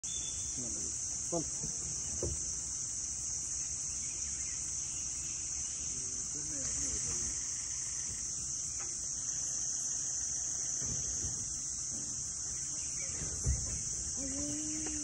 Sambar Deer
The sambar is a large deer native to the Indian subcontinent and Southeast Asia that is listed as a vulnerable species on the IUCN Red List since 2008. Populations have declined substantially due to severe hunting, local insurgency, and industrial exploitation of habitat. @XubhoDin